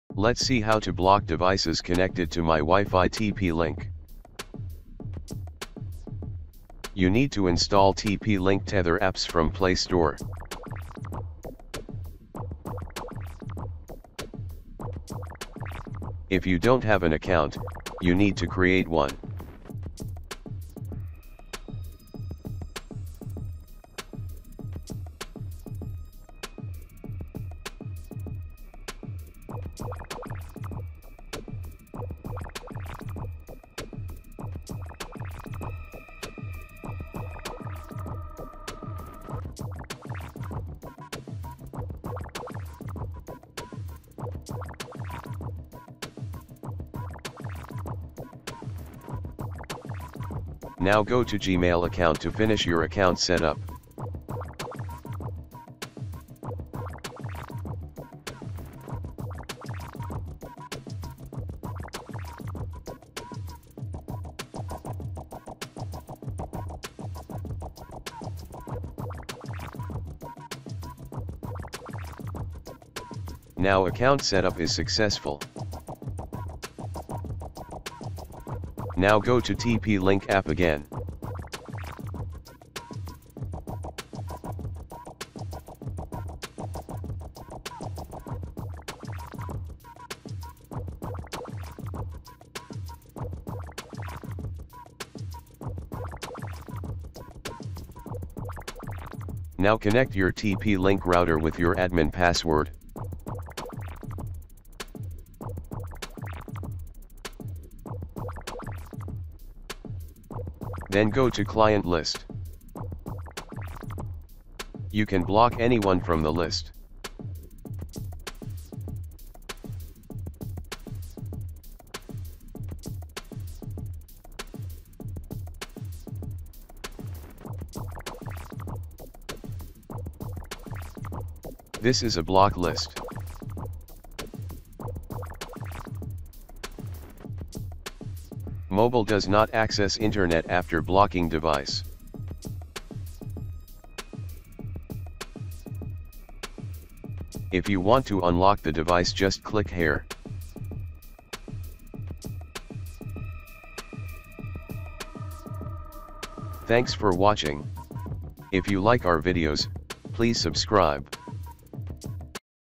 How to block devices connected to my wifi tp link
Let's see how to block unknown wifi user on TP-Link router